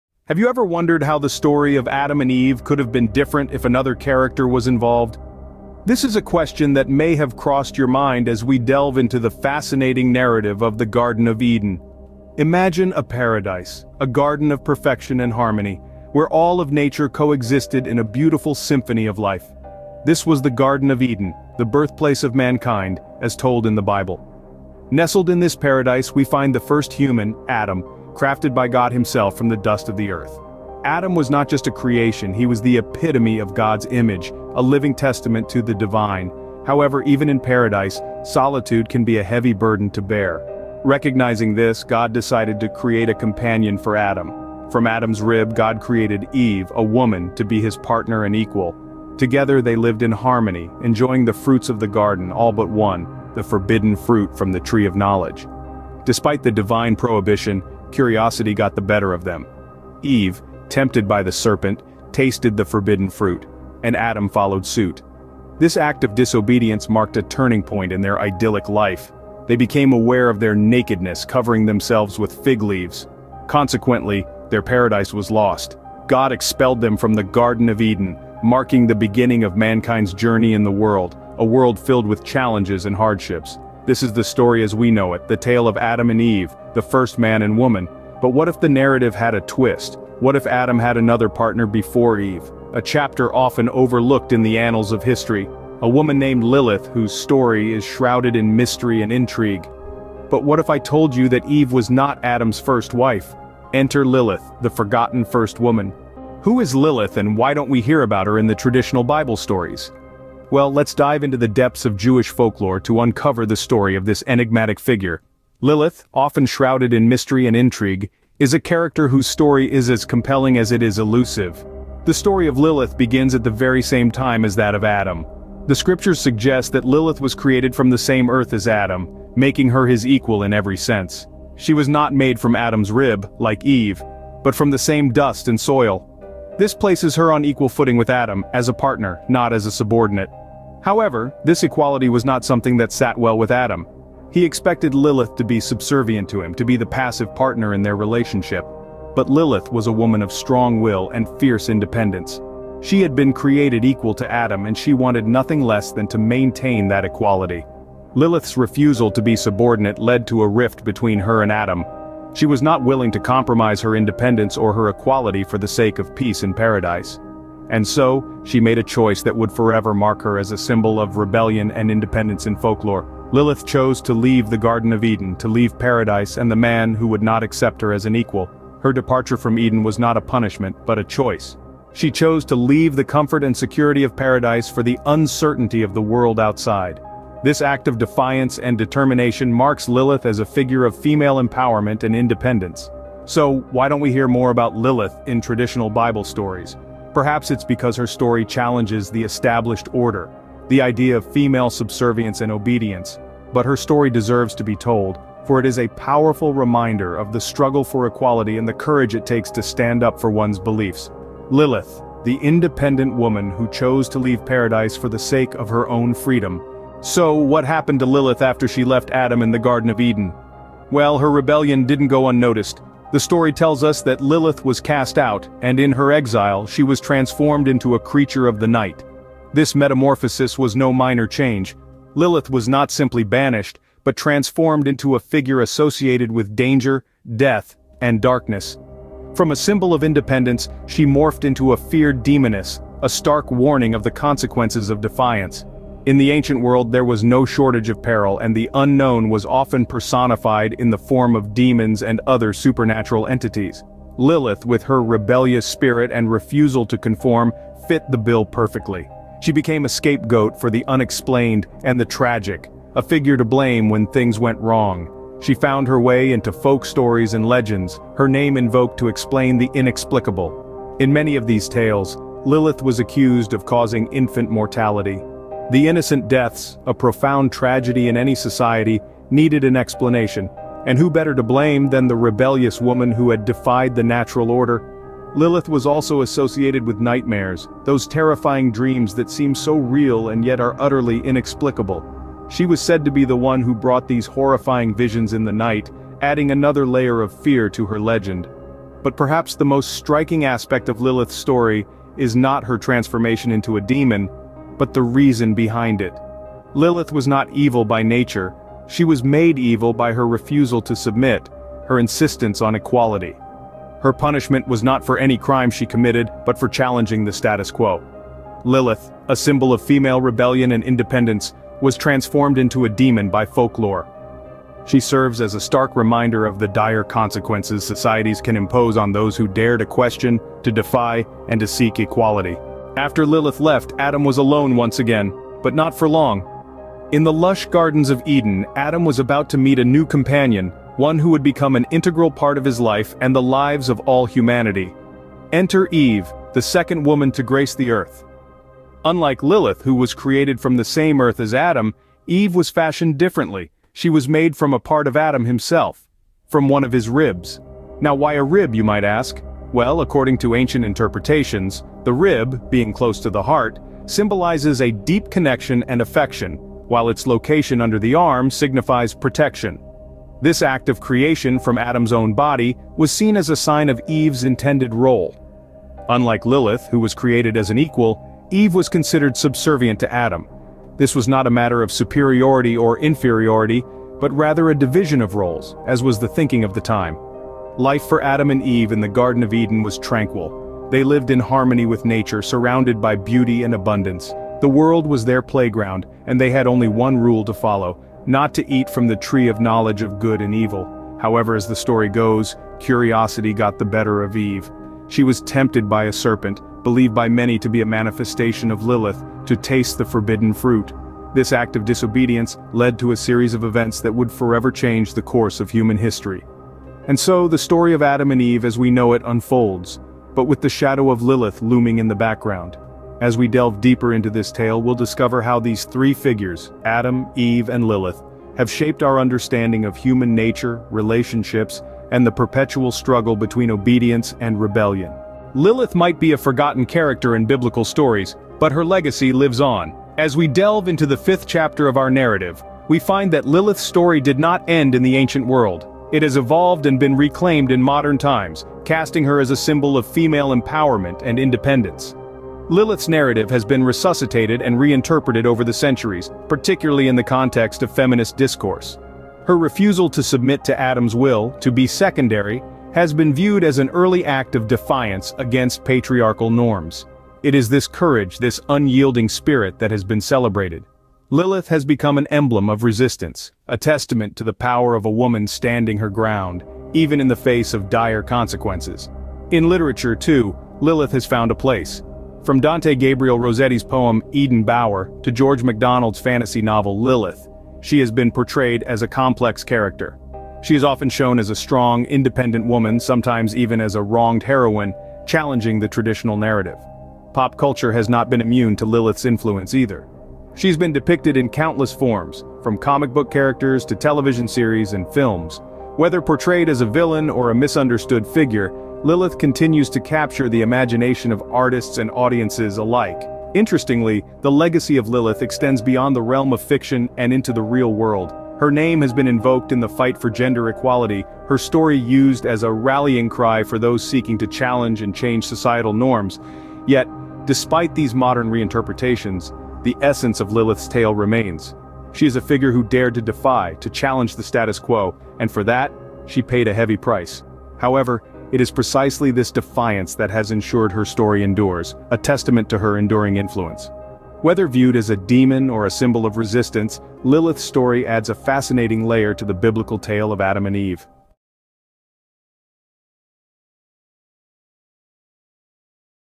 mythological story of adam, eve and lilith
Have you ever wondered how the story of Adam and Eve could have been different if another character was involved? This is a question that may have crossed your mind as we delve into the fascinating narrative of the Garden of Eden. Imagine a paradise, a garden of perfection and harmony, where all of nature coexisted in a beautiful symphony of life. This was the Garden of Eden, the birthplace of mankind, as told in the Bible. Nestled in this paradise, we find the first human, Adam, crafted by God himself from the dust of the earth. Adam was not just a creation, he was the epitome of God's image, a living testament to the divine. However, even in paradise, solitude can be a heavy burden to bear. Recognizing this, God decided to create a companion for Adam. From Adam's rib, God created Eve, a woman, to be his partner and equal. Together, they lived in harmony, enjoying the fruits of the garden, all but one - the forbidden fruit from the Tree of Knowledge. Despite the divine prohibition, curiosity got the better of them. Eve, tempted by the serpent, tasted the forbidden fruit, and Adam followed suit. This act of disobedience marked a turning point in their idyllic life. They became aware of their nakedness, covering themselves with fig leaves. Consequently, their paradise was lost. God expelled them from the Garden of Eden, marking the beginning of mankind's journey in the world, a world filled with challenges and hardships. This is the story as we know it, the tale of Adam and Eve, the first man and woman. But what if the narrative had a twist? What if Adam had another partner before Eve? A chapter often overlooked in the annals of history. A woman named Lilith, who's story is shrouded in mystery and intrigue. But what if I told you that Eve was not Adam's first wife? Enter Lilith, the forgotten first woman.